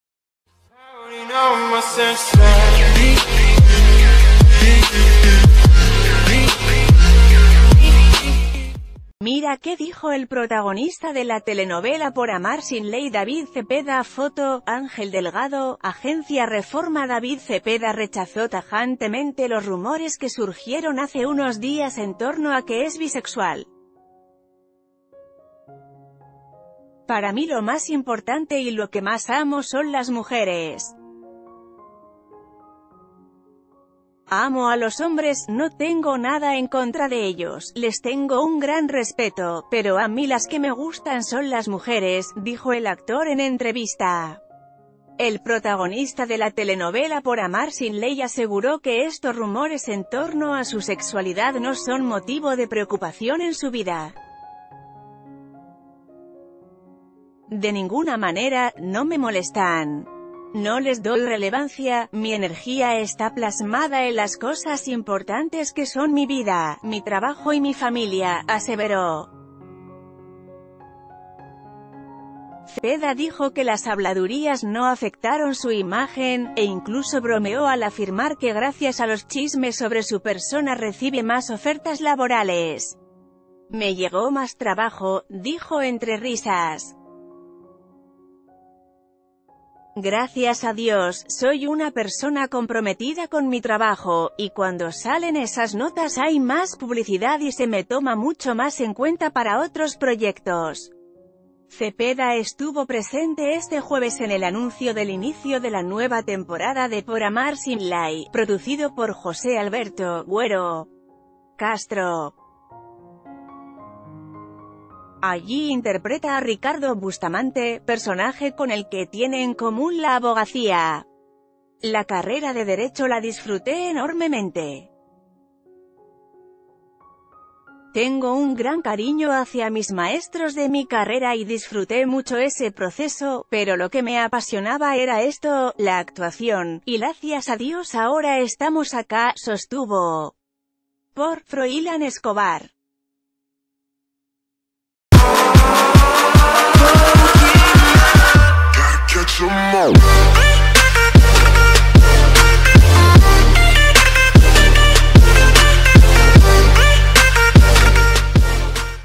Esto dijo David Zepeda sobre los rumores de su supuesta bisexualidad
Mira qué dijo el protagonista de la telenovela Por Amar Sin Ley David Zepeda Foto: Ángel Delgado / Agencia Reforma David Zepeda rechazó tajantemente los rumores que surgieron hace unos días en torno a que es bisexual. “Para mí lo más importante y lo que más amo son las mujeres. Amo a los hombres, no...